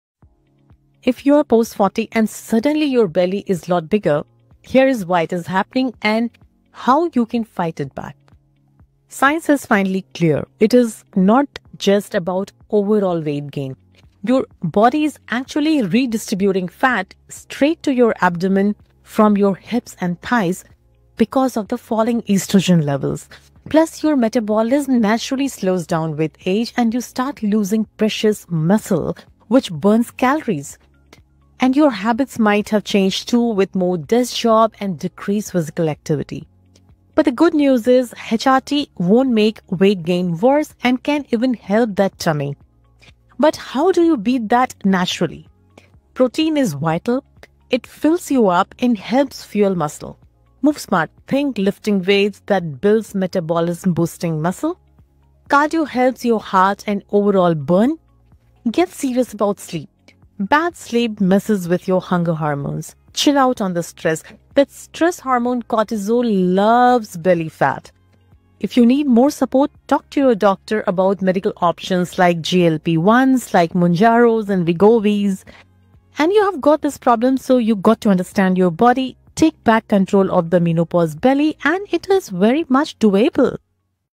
Doctor Explains Why’s And How’s Of Menopausal Belly menopause menopausalbelly Over 40 and
Doctor Explains Why’s And How’s Of Menopausal Belly

Over 40 and suddenly your belly’s way bigger? Here’s why – and what you can actually do about it.

✅ What’s going on:
Science is finally clear! It’s not just general weight gain. As estrogen drops, your body starts shifting fat from your hips and thighs straight onto your tummy. Your metabolism slows down, you lose muscle (which burns calories!), and often your habits change—more sitting, less moving.

💡 Good news:
HRT doesn’t make weight gain worse. In fact, it can help reduce belly fat.

✅ How to fight back naturally:
🥩 Eat more protein. It keeps you full and helps protect muscle.
🏋️ Lift weights. Strength training builds metabolism-boosting muscle.
🚶 Get your steps in. Cardio supports your heart and burns extra energy.
😴 Fix your sleep. Poor sleep messes with hunger hormones.
🧘 Manage stress. High cortisol loves to store fat around your waist.

Chat with your doctor about medical options like GLP-1s.

You’ve got this. Understand your body and take back control of that menopause belly! 💪